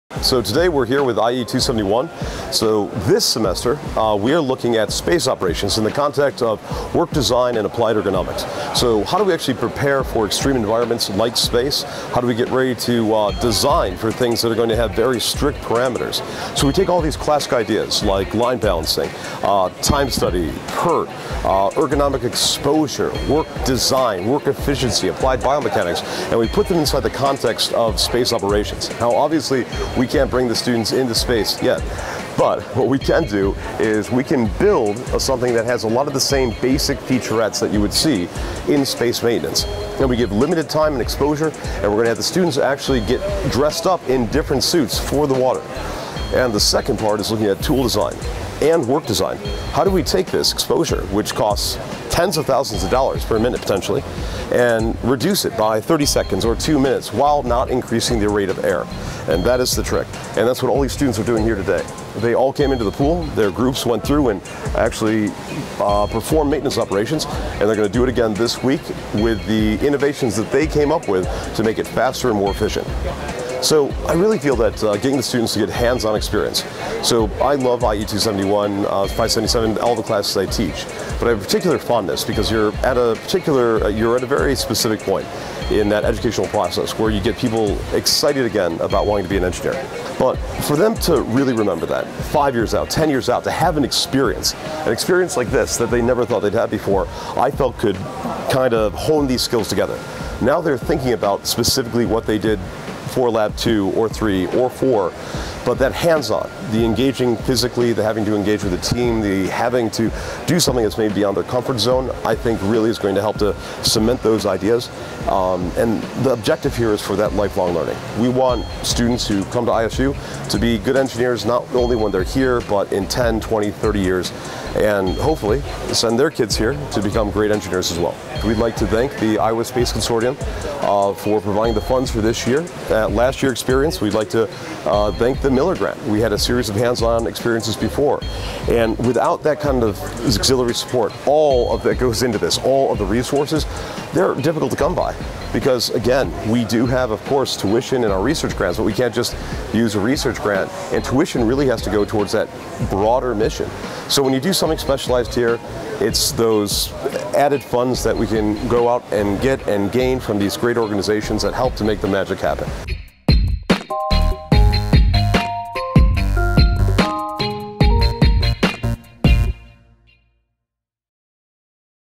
IE 271 at State Pool
Students in Industrial and Manufacturing Systems Engineering Associate Professor Rick Stone's I E 271: Applied Ergonomics and Work Design course got out of the classroom and into the pool for some real hands-on learning. The pool was meant to simulate a zero gravity environment and students performed maintenance activities on a structure that was meant to resemble parts of a space station.